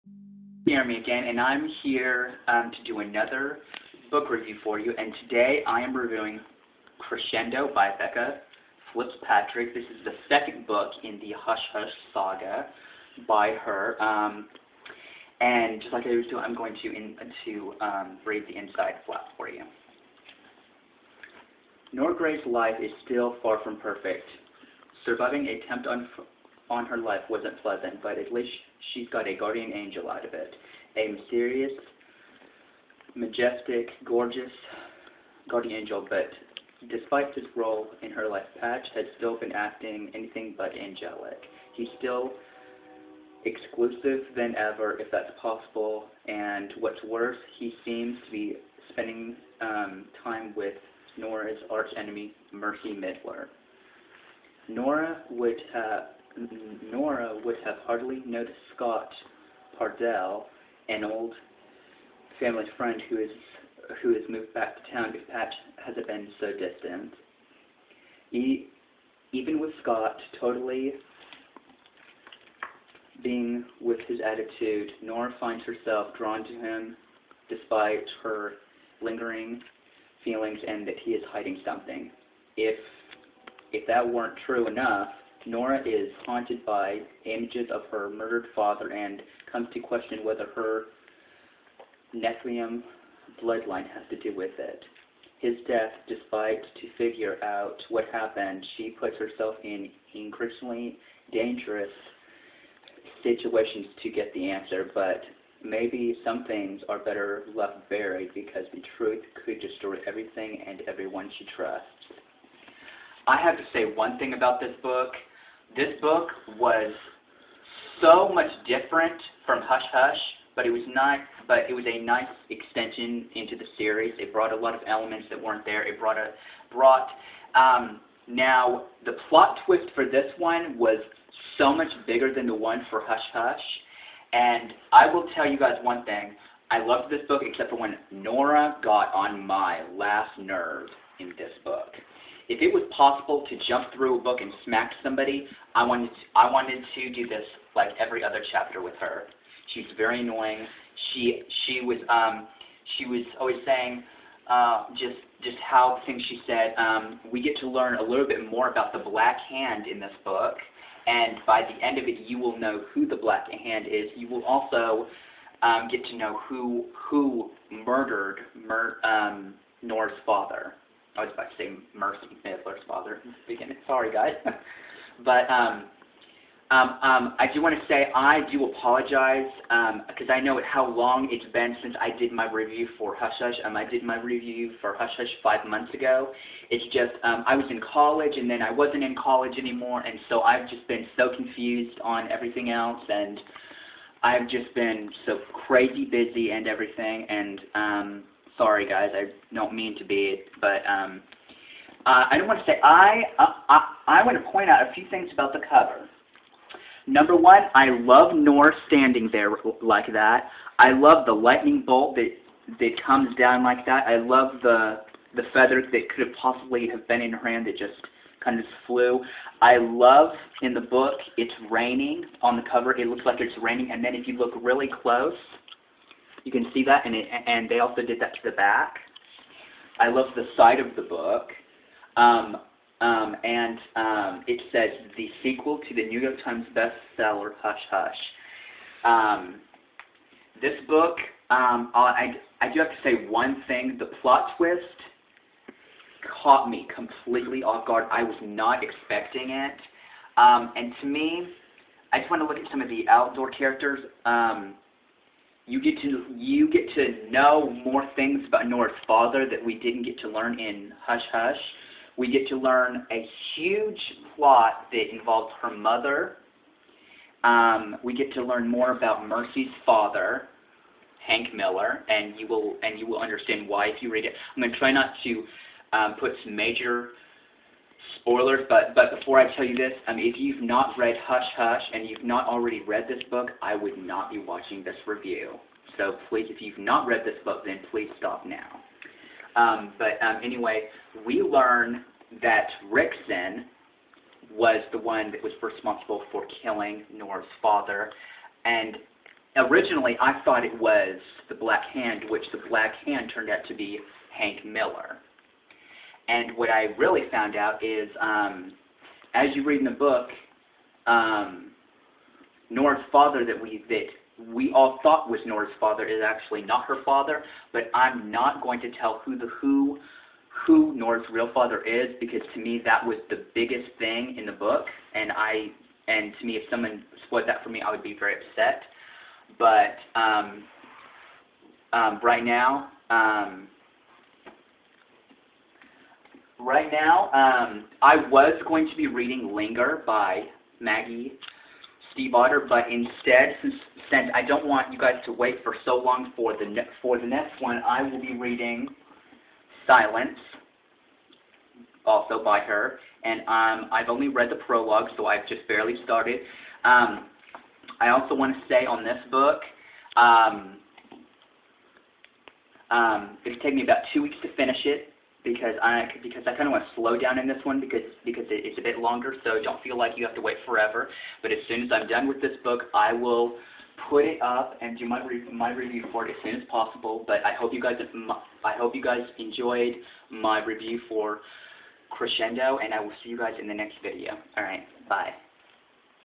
Crescendo by becca Fitzpatrick book review
Book 2 in the hush, hush series

Nora Grey's life is still far from perfect. Surviving an attempt on her life wasn't pleasant, but at least she got a guardian angel out of it. A mysterious, magnetic, gorgeous guardian angel. But, despite his role in her life, Patch has been acting anything but angelic. He's more elusive than ever (if that's possible) and what's worse, he seems to be spending time with Nora's arch-enemy, Marcie Millar.

Nora would have hardly noticed Scott Parnell, an old family friend who has moved back to town, if Patch hadn't been acting so distant. Even with Scott's totally infuriating attitude, Nora finds herself drawn to him - despite her lingering feeling that he's hiding something.

If that weren't enough, Nora is haunted by images of her murdered father, and comes to question whether her Nephilim bloodline has anything to do with his death. Desperate to figure out what happened, she puts herself in increasingly dangerous situations to get the answer. But maybe some things are better left buried, because the truth could destroy everything - and everyone - she trusts.